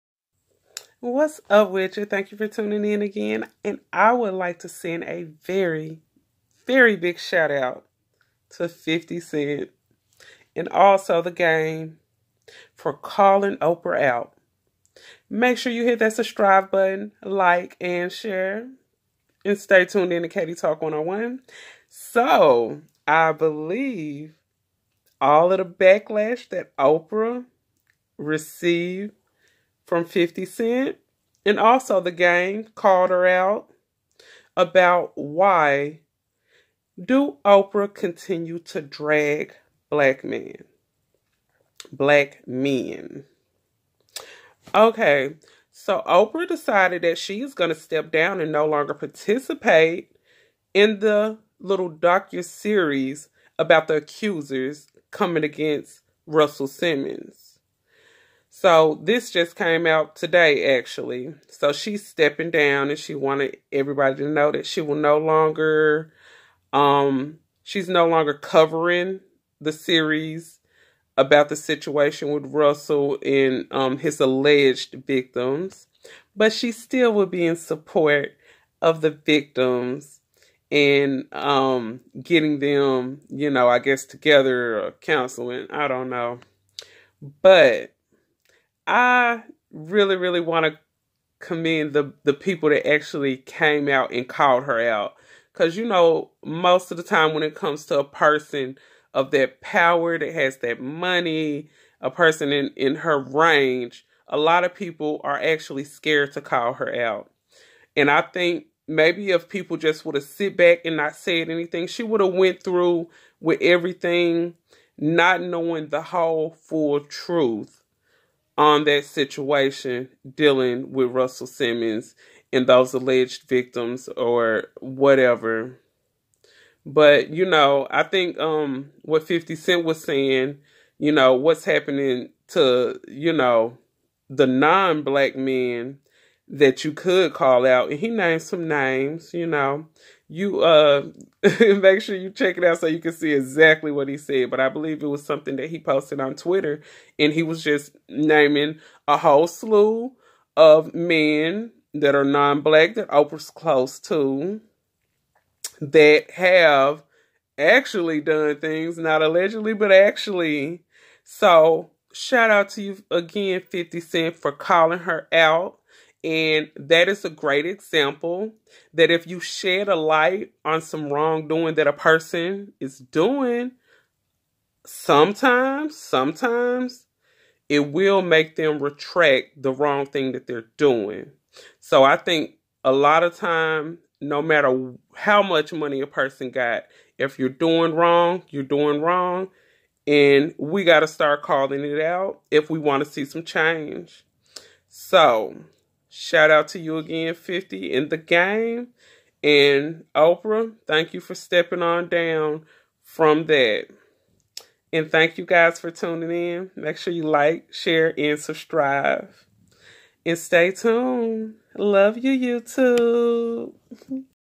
Bails Out Of The MeToo Documentary On Russell Simmons!!
Oprah Winfrey steps down from meto documentary against Russell Simmons after receiving backlash from 50 Cent and the rapper The Game.

CHECK OUT LINK 👇👇👀

CHECK ME OUT ON ANCHOR 👇👀